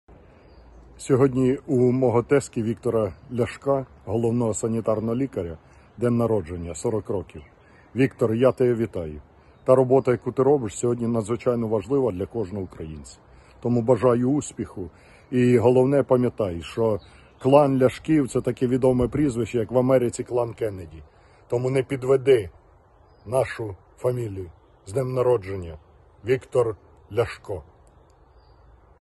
Ляшко вітає Ляшка з днем народження
Лідер Радикальної партії Олег Ляшко привітав із днем народження головного санітарного лікаря країни Віктора Ляшка.